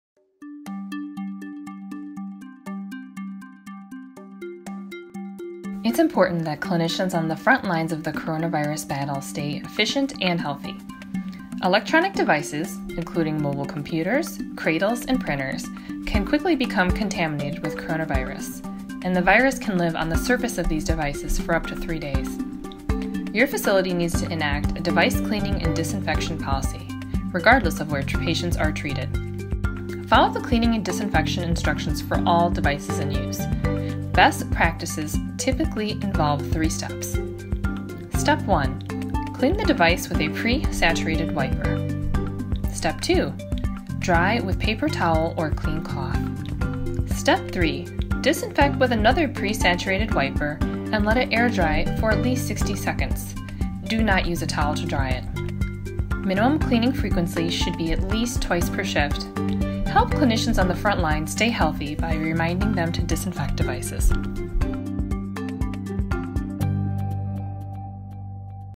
Infection Prevention for Alternative Care Sites (Korean Version) | Zebra
When medical facilities are overwhelmed with an influx of patients during an infection outbreak, they set up alternative care facilities, such as medical tents or temporary hospital sites.
These alternative care facilities increase bed capacity and help alleviate the pressures on intensive care and emergency room facilities in traditional hospitals. They typically offer only the essential services needed to combat an infection outbreak, with fast-paced and hectic environments that can lead to increased errors in patient identification and treatments.

To learn more about Zebra's response to COVID-19, please click the following